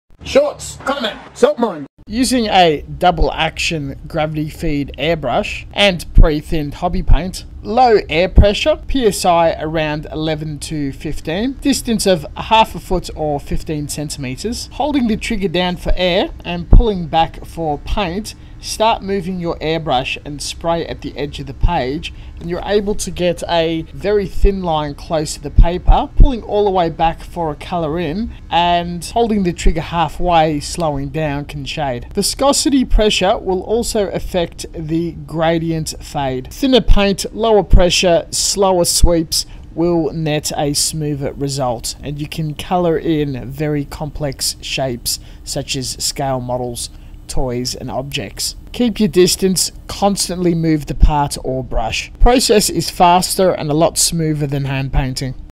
how to airbrush plamo
how to airbrush gunpla! this video will demonstrate and visually explain adding paint to the cup, regulating the air compressor and spraying model parts. Intent to use for your hobby, art works, plastic scale model kits, gunpla and gundam models, GK anime resin garage kits, figures, miniatures, toys and 3d prints. In easy guide and tutorial form for beginners.

Projects you can use and mix but not limited too:
double action
single action
spray gun
gravity feed
siphon feed